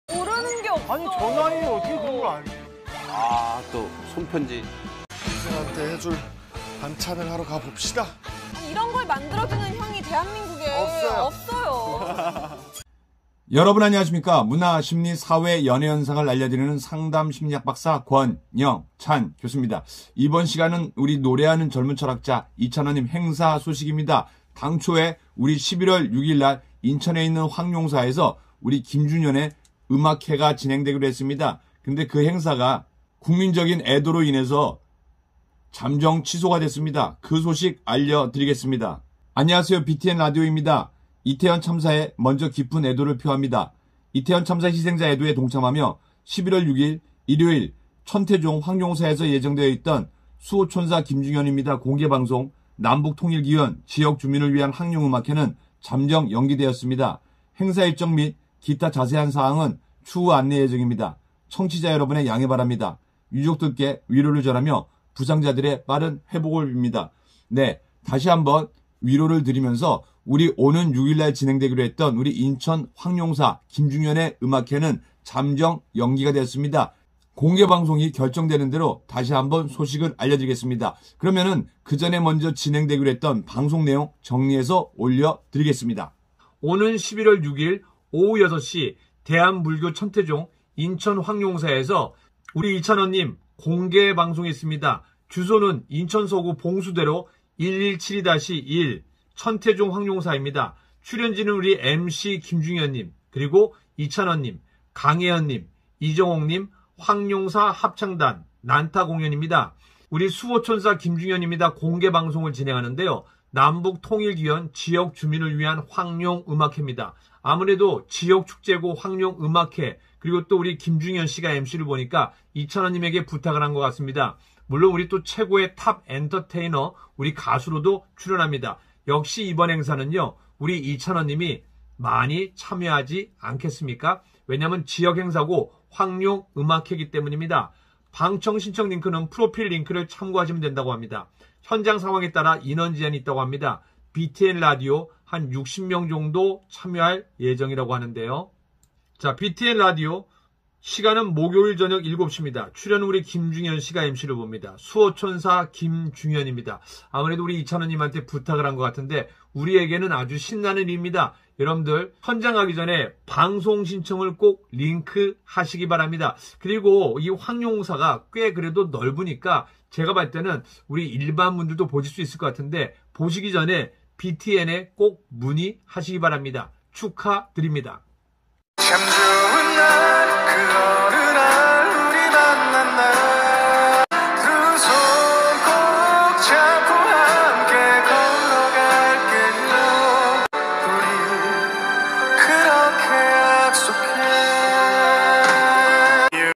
[이찬원 6일 인천 황룡사 음악회 취소 연기| BTN 황룡사 김중연 공개방송 취소 및 연기 안내]
이찬원 6일 인천 황룡사 음악회 취소 및 연기 안내